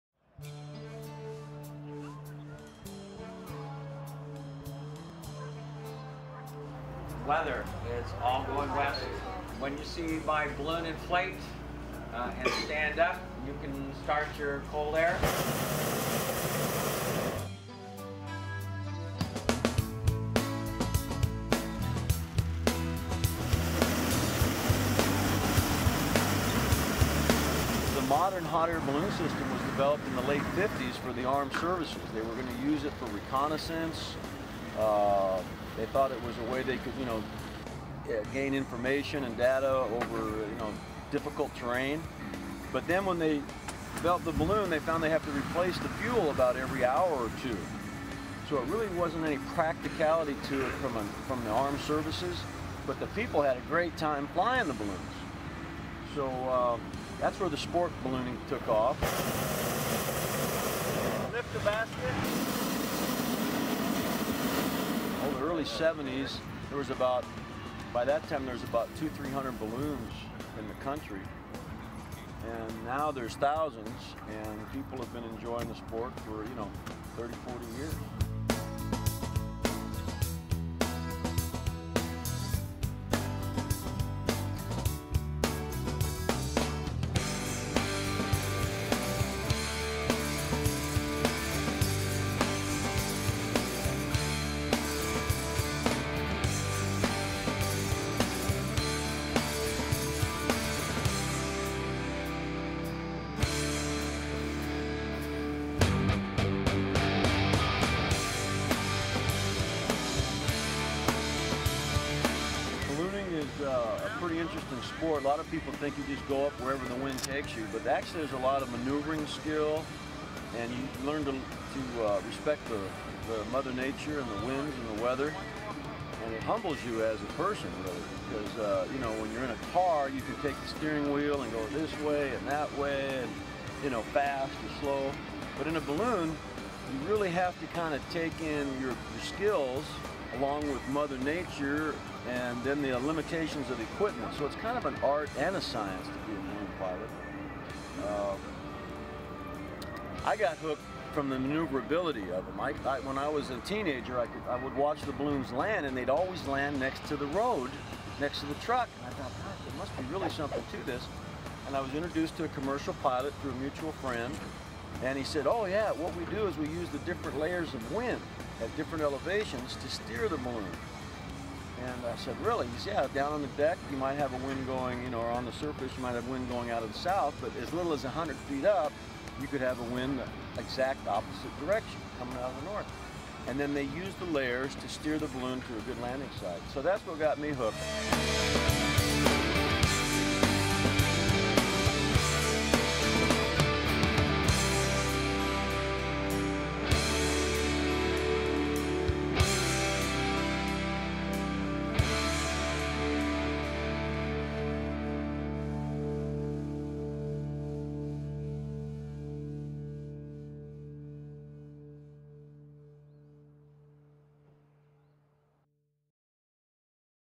Santa Paula Citrus Classic 2012
Annual Hot Air Balloon Festival held in Santa Paula,ca usually in late July or early August